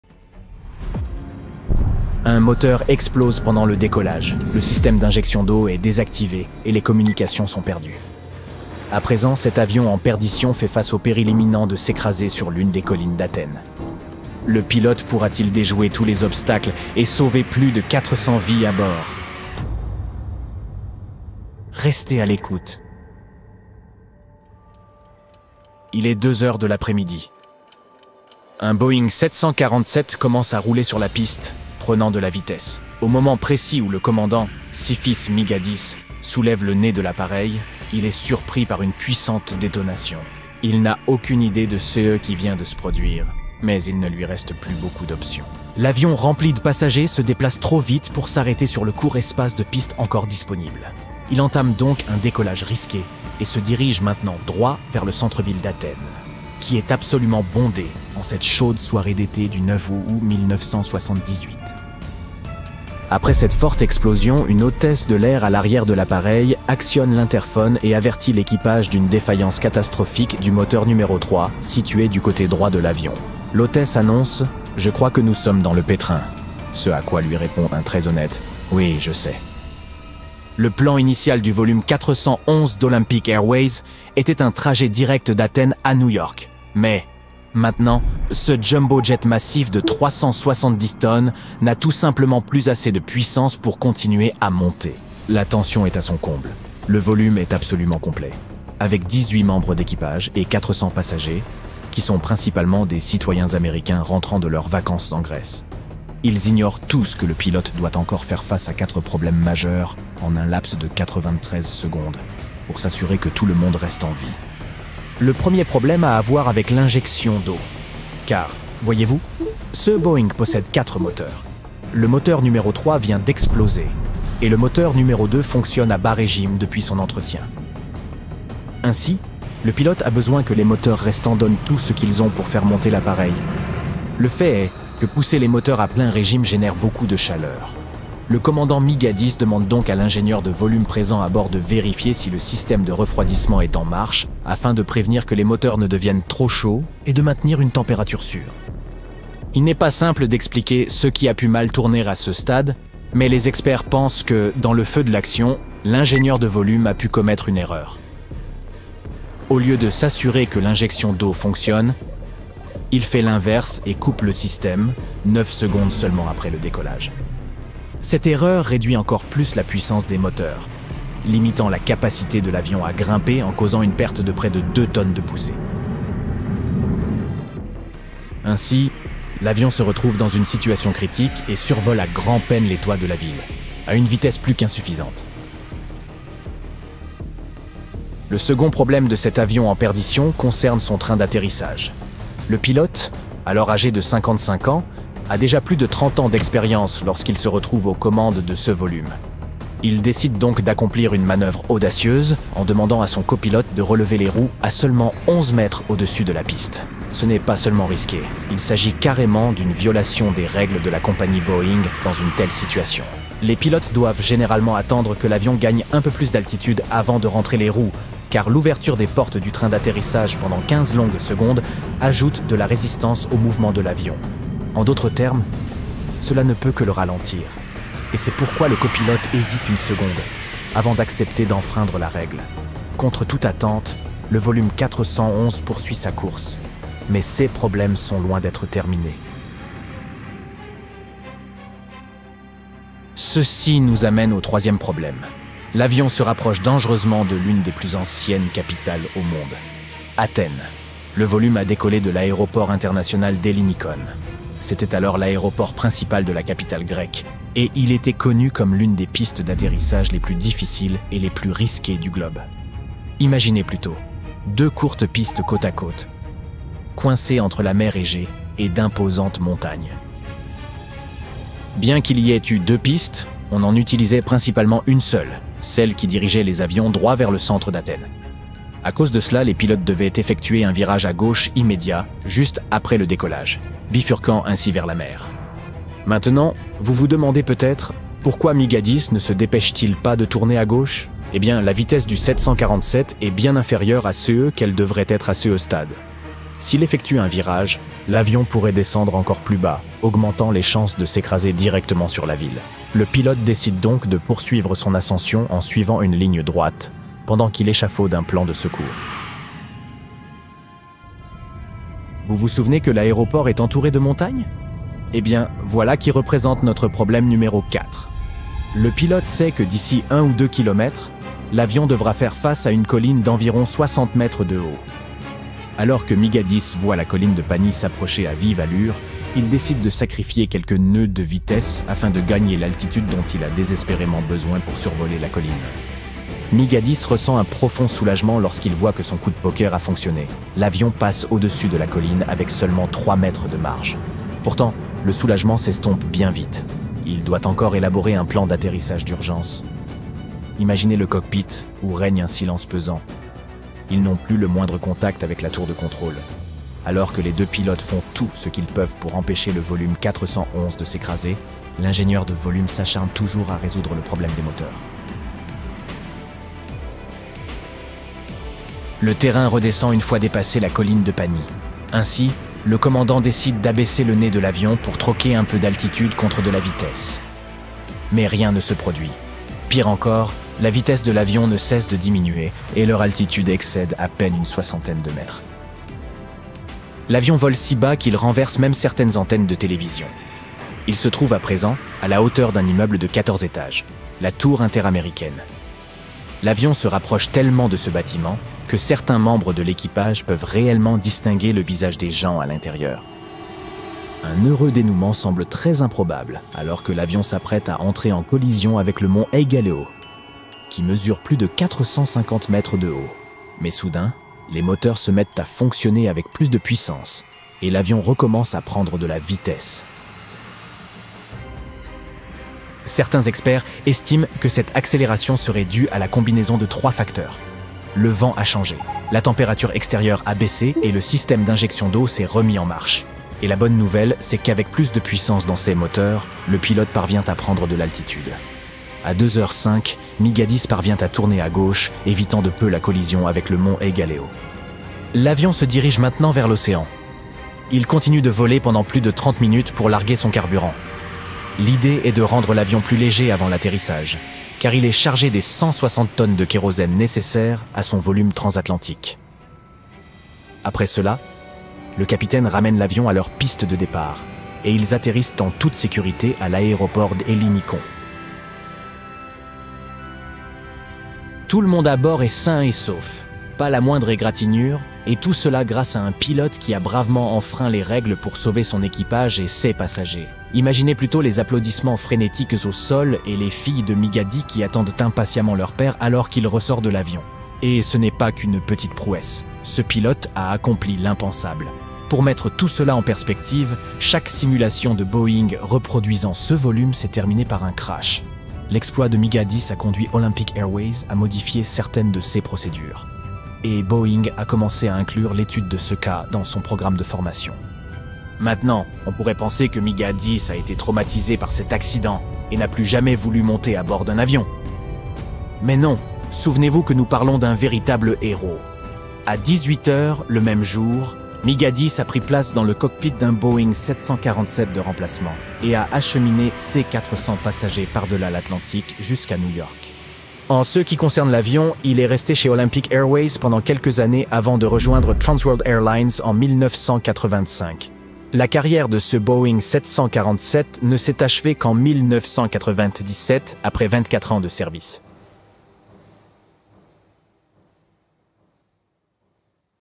Un Avion Qui a Failli S’écraser Sur Athènes
Vous n'allez pas croire ce qui s'est passé : il y avait cet avion qui volait vers Athènes, non ? Et il était en train d'atterrir quand les choses sont devenues vraiment risquées. On parle de niveaux d'intensité "presque écrasés". Heureusement, les pilotes ont réussi à réaliser des manœuvres épiques et à éviter le désastre à la dernière seconde. Pouvez-vous imaginer le chaos si cela s'était produit ? Heureusement, tout le monde s'en est sorti indemne, mais bon sang, parlez d'un moment qui vous arrête le cœur !